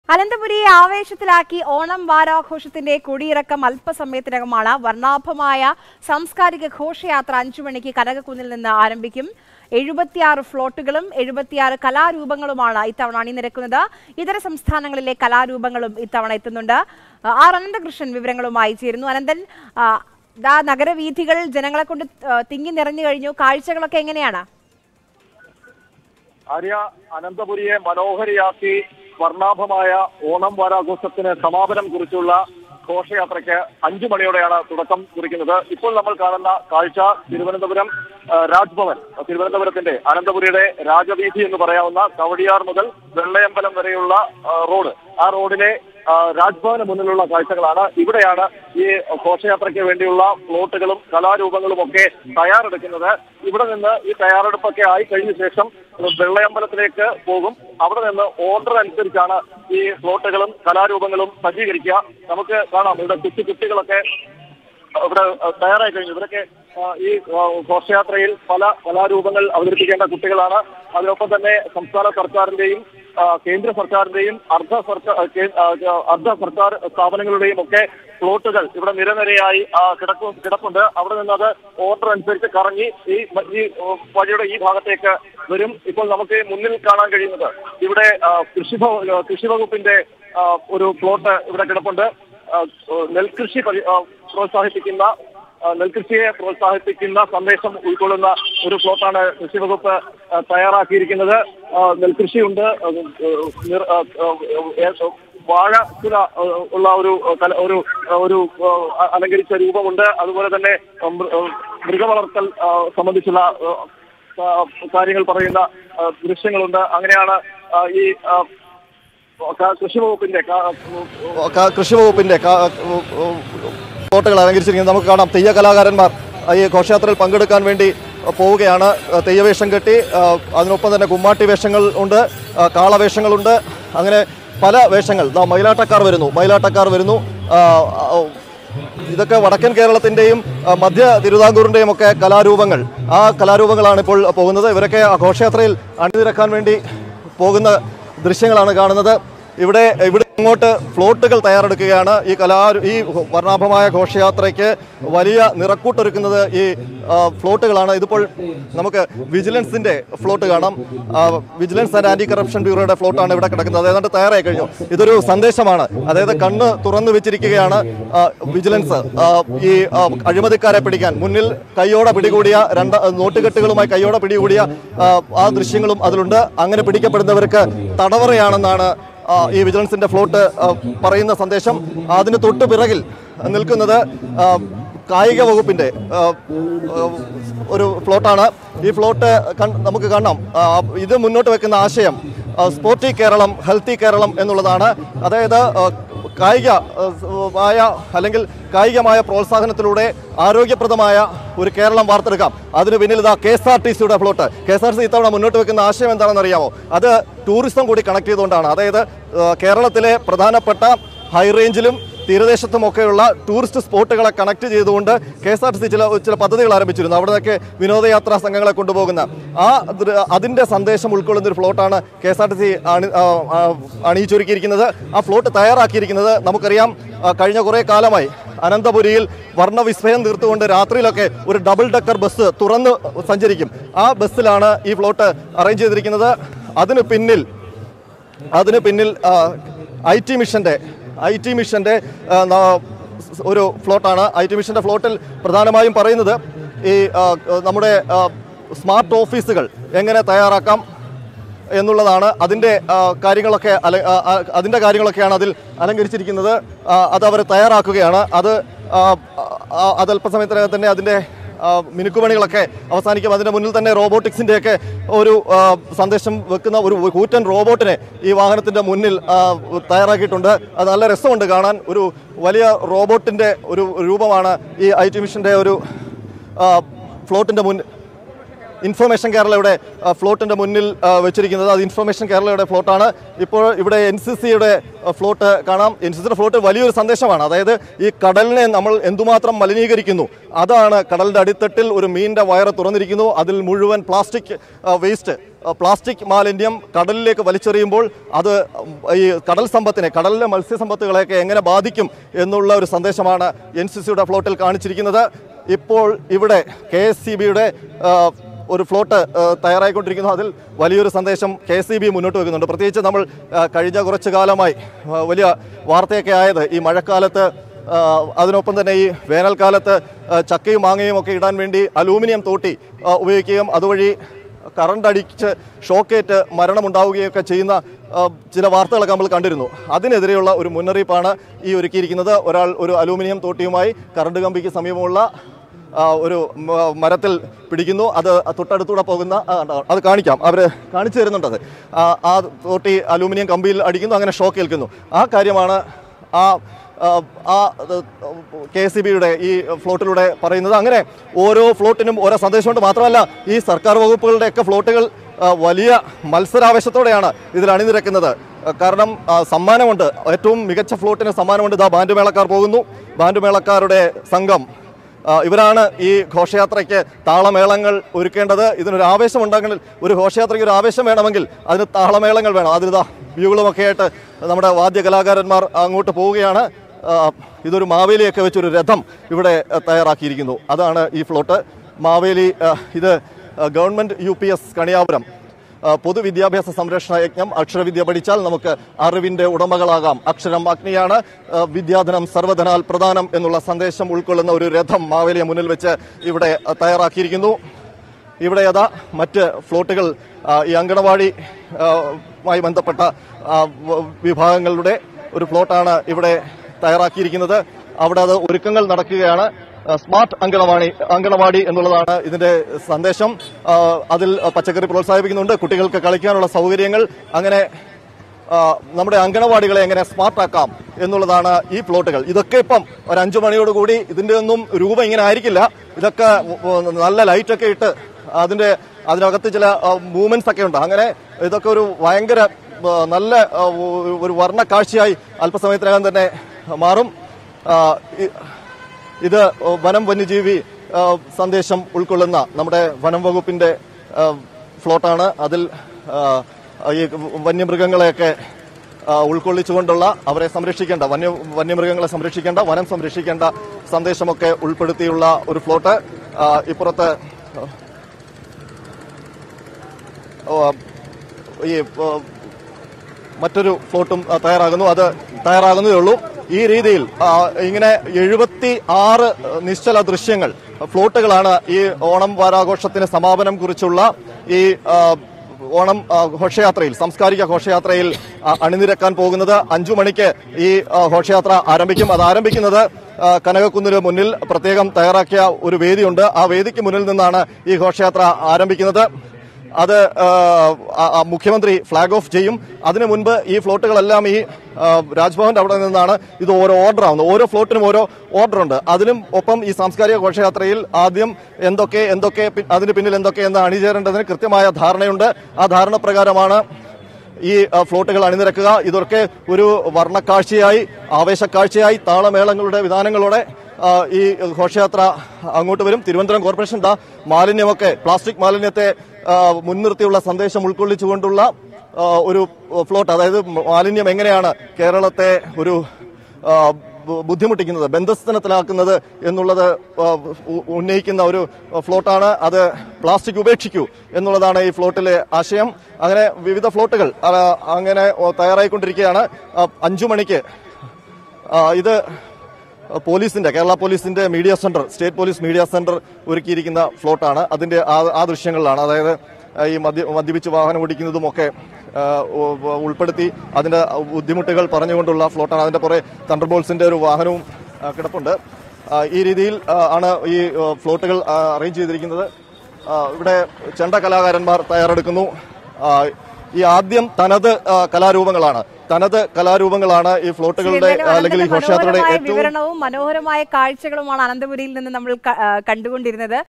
കളറായി അനന്തപുരി..! ഓണം ഘോഷയാത്ര അൽപ്പസമയത്തിനകം | Mathrubhumi News
അനന്തപുരിയെ ആവേശത്തിലാക്കി ഓണം വാരാഘോഷത്തിന്റെ കൊടിയിറക്കം അൽപസമയത്തിനകം. വർണാഭമായ സാംസ്കാരിക ഘോഷയാത്ര 5 മണിക്ക് കനകക്കുന്നിൽ നിന്ന് ആരംഭിക്കും.

Happy viewing!